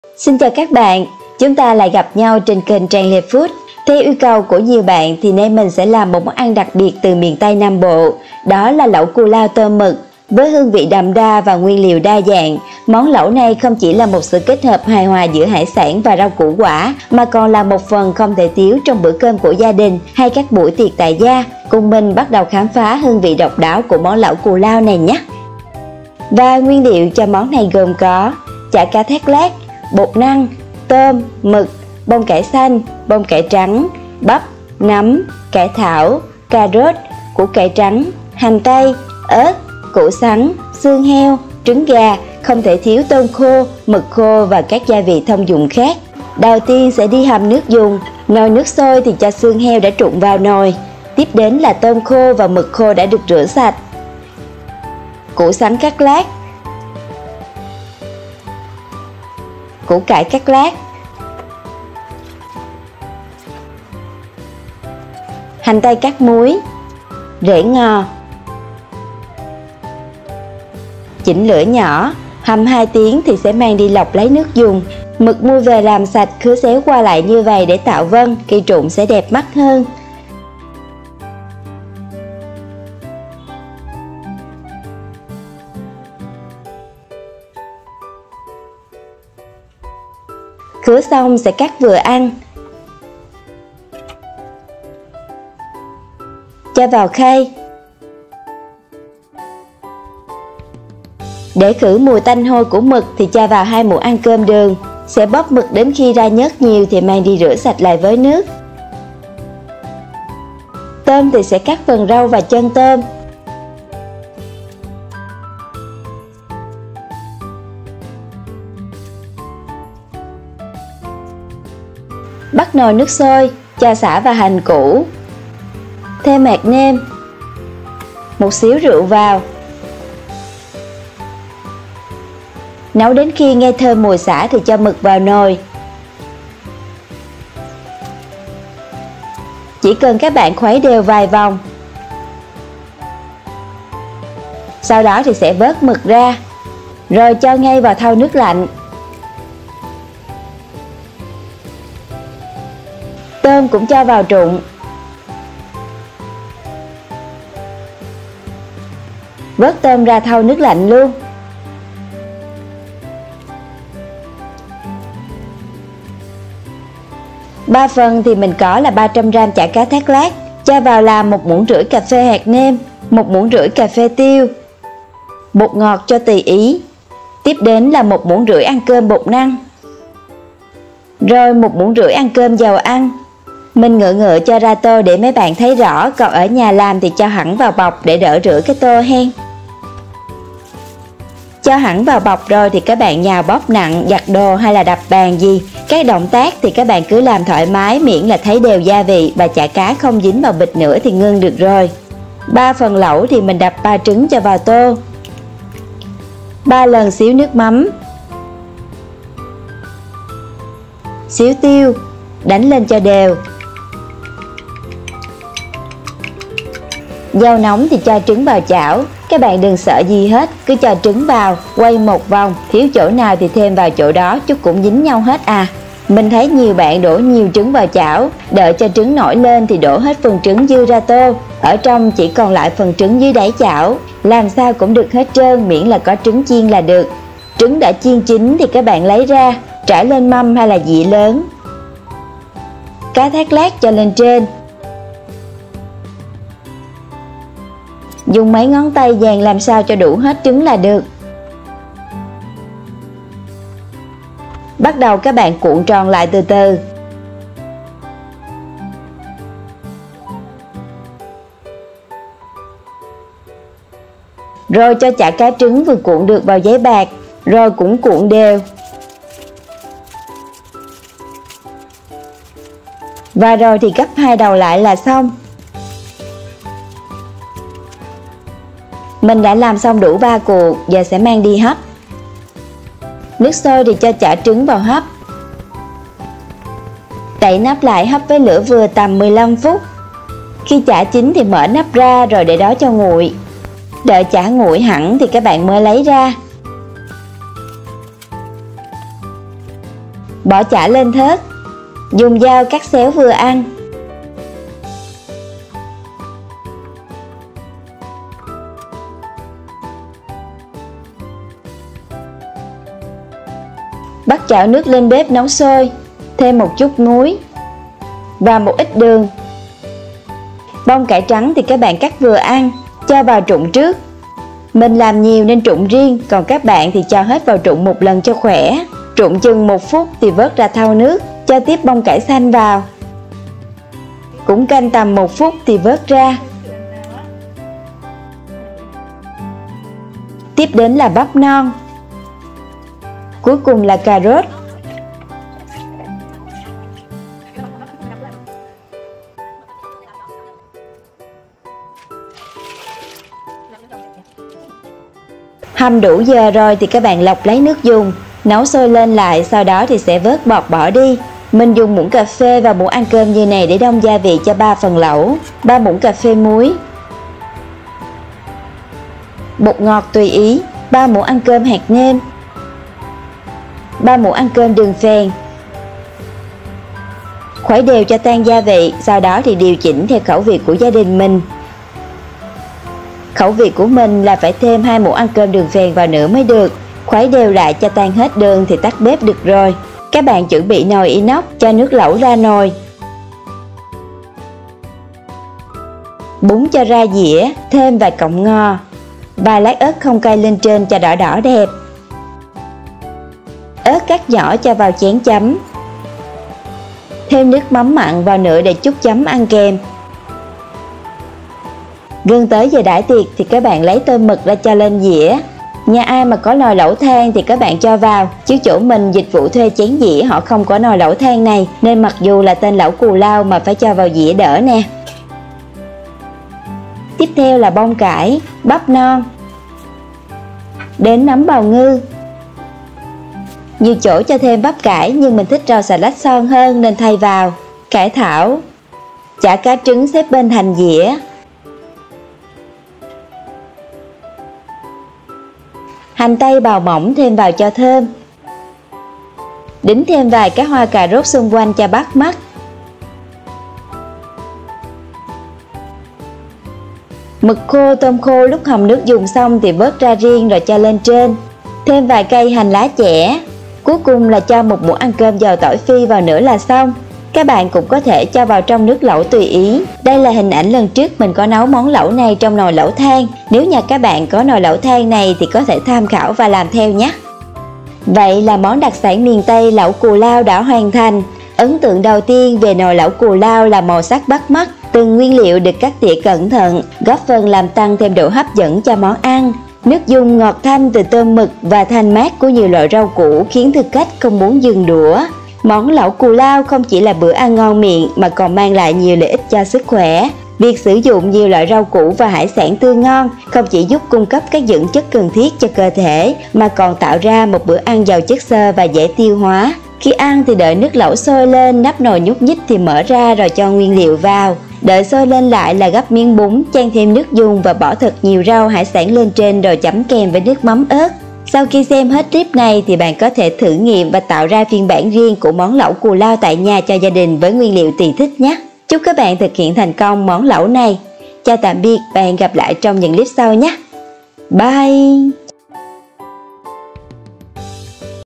NẤU ĂN ĐÃI TIỆC - TRẢ MÓN LẨU CÙ LAO CHO BẠN NÀO YÊU CẦU ĐÂY
Xin chào các bạn, chúng ta lại gặp nhau trên kênh Trang Le Food! Theo yêu cầu của nhiều bạn thì nay mình sẽ làm một món ăn đặc biệt từ miền Tây Nam Bộ - đó là lẩu cù lao tôm mực. Với hương vị đậm đà và nguyên liệu đa dạng, món lẩu này không chỉ là một sự kết hợp hài hòa giữa hải sản và rau củ quả mà còn là một phần không thể thiếu trong bữa cơm gia đình và các buổi tiệc tại gia. Hãy cùng mình bắt đầu khám phá hương vị độc đáo của lẩu cù lao nhé
Nguyên liệu cho món này gồm có: chả cá thác lác, bột năng, tôm, mực, bông cải xanh, trắng, bắp, nấm, cải thảo, cà rốt, củ cải trắng, hành tây, ớt, củ sắn, xương heo, trứng gà, tôm khô, mực khô và các loại gia vị thông dụng khác
CÔNG THỨC GIA VỊ CÓ TRONG CLIP NHÉ CÁC BẠN